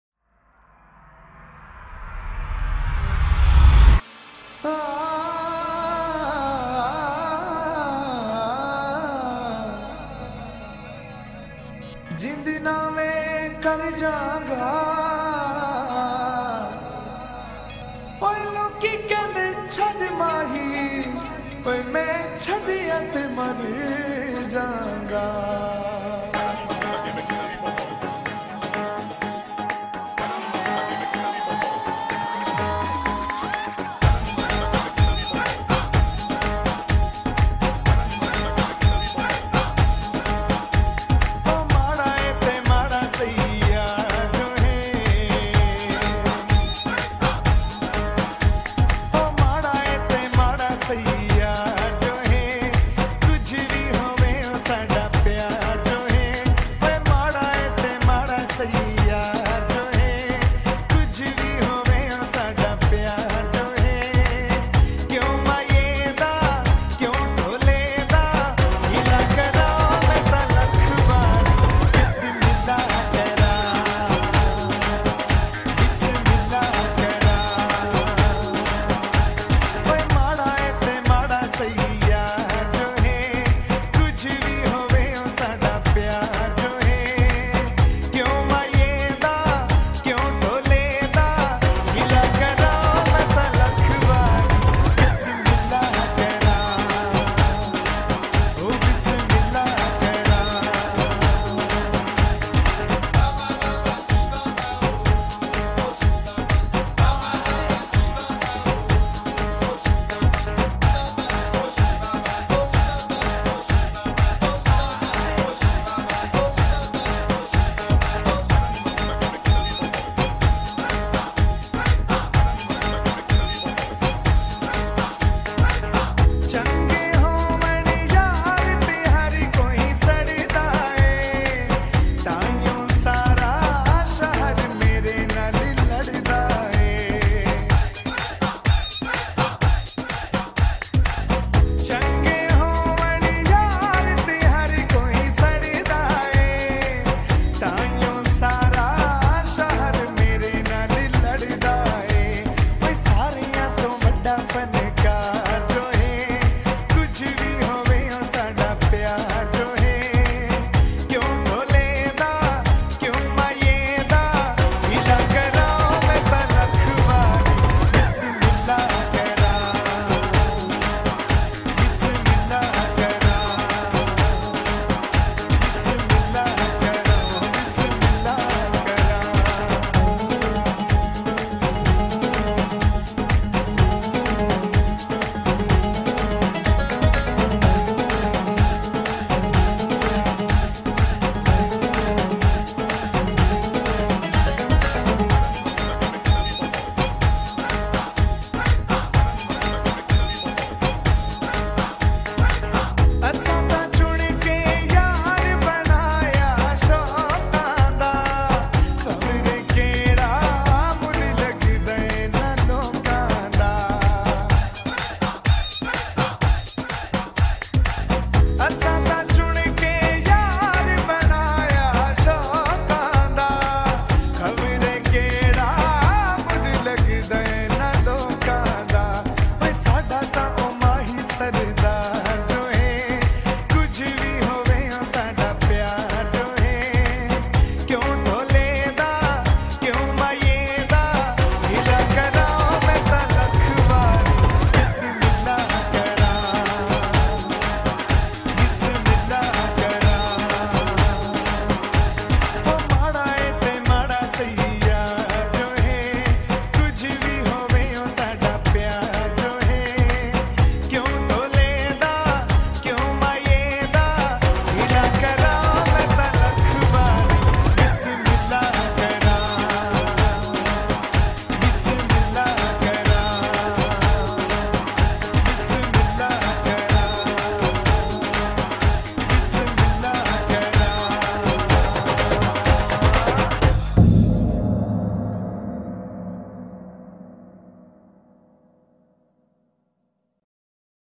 A Very Pakistani Traditional Wedding of Fahima & Khurram
Bismillah Karan
Produced by: EBM Studios
Story by: Farhan | Mustafa | Kristina
In association with EBM Photography